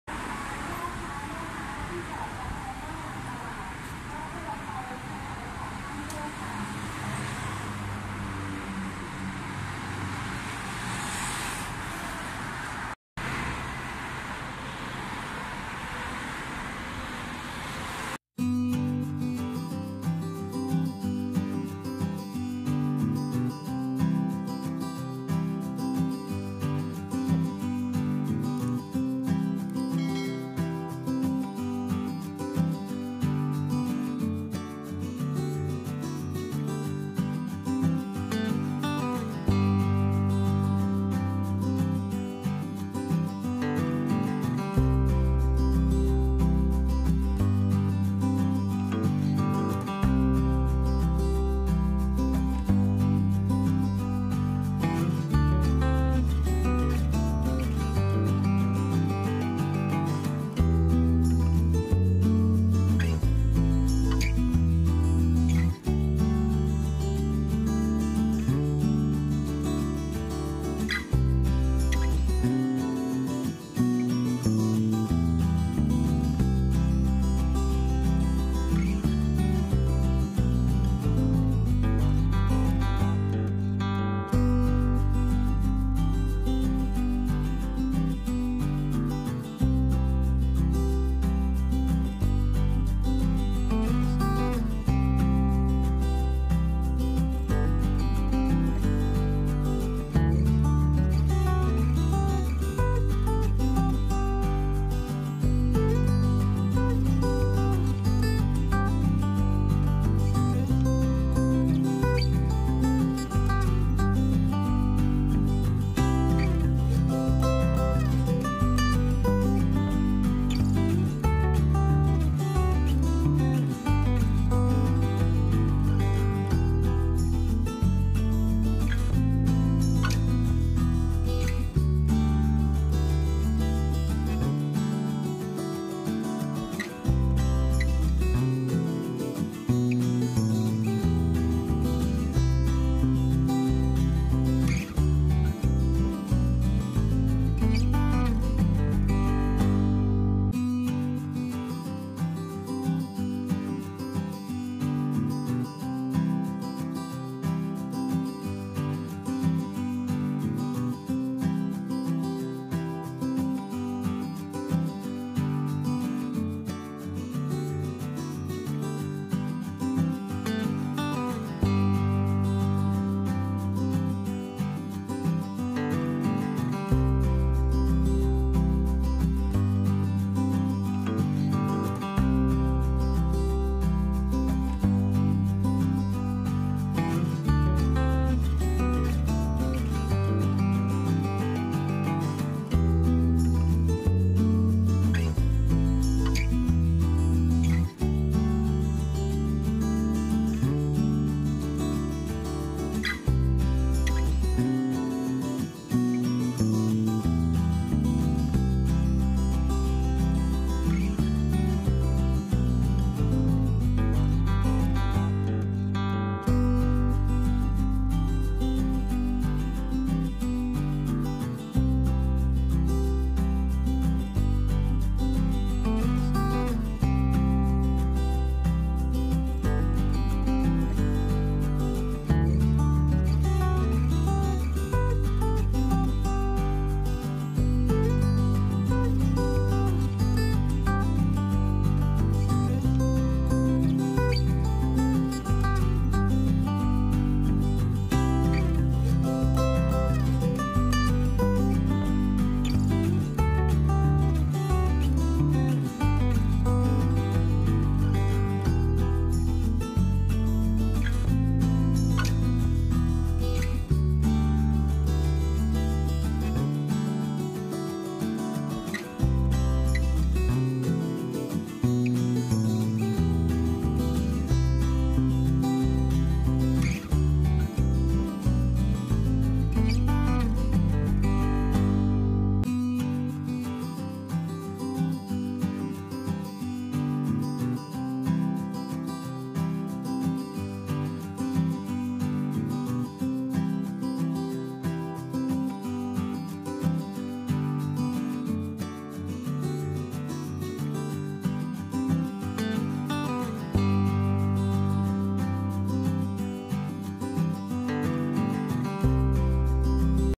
Thai Coconut Pancake l Khanom Baa Bin thaidessert Bangkok Street Food
Thai Coconut Pancake, Khanom Baa Bin thaidessert Bangkok Street Food, Thai Street Food
Thai Coconut Pancakes Shop
Place : Meet Suk Market Rama II Road, Bangkok
Open Daily
Time : 1.00 pm. - 8.30 pm.